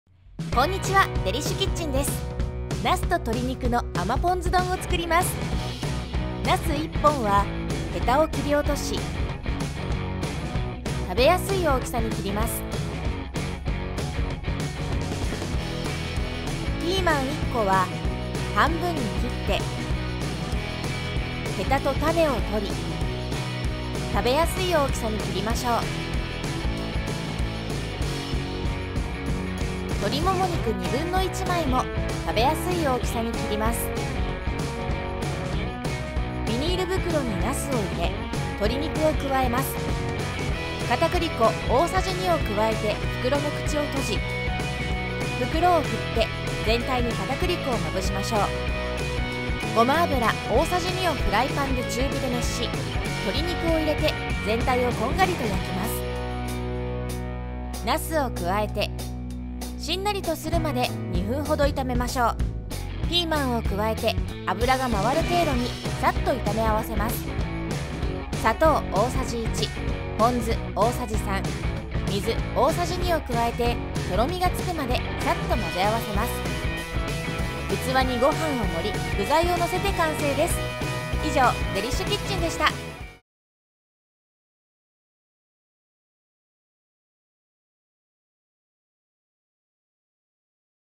じゅわっとジューシー♡なすと鶏肉の甘ポン酢丼｜How To Make Lotus and fried chicken with sweet vinegar
アプリのダウンロードはこちら

なすと鶏肉は少なめの油で炒めることで、表面に膜を作り、中はしっとりでジューシーな仕上がりになります。今晩の一品にいかがでしょうか？

・ごはん　　　どんぶり2杯
・鶏もも肉　　1/2枚(125g)
☆甘酢たれ
・砂糖　　　　大さじ1
・ポン酢　　　大さじ3

■手順
⑴ なすはへたを切り落とし、食べやすい大きさに切る。ピーマンは半分に切り、へたと種を取る。食べやすい大きさに切る。
⑵ 鶏肉は食べやすい大きさに切る。
⑶ ビニール袋になす、鶏肉、片栗粉を入れて袋の口を閉じて振り、全体に片栗粉をまぶす。
⑷ フライパンにごま油を入れて熱し、鶏肉を入れ、中火で全体がこんがりと焼けたらなすを加え、しんなりとするまで2分程炒める。ピーマンを加えて油が回る程度にさっと炒め合わせ、☆を加えてとろみがつくまでさっと混ぜ合わせる。
⑸ 器にごはんを盛り、4をのせる（ごはんは温かいごはんを使ってください。）

少ない油でジューシー!ささみの揚げ焼きチキン南蛮

食材はお肉とチーズだけ！チーズタッカルビ風甘辛チキンソテー

みそ風味でコクもアップ！なすと鶏のこっ照り焼き丼